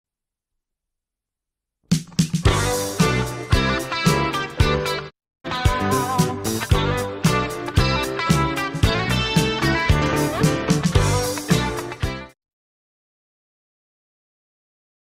Happy 44TH Birthday To Kristen Schaal With Robchuckle And Friends 24TH January! ;D
Hello chucklings it's Robchuckle here and this is the happy birthday to the American actress us kristen schaal and she plays the tv shows and the movies of Bob's Burgers and The Last Man On Earth and BoJack Horseman Gravity Falls and she plays mabel pines and she turned 44 today and have a brilliant day kristen love from XXMeXX and don't forget to like and subscribe and enjoy the chuckle videos? :)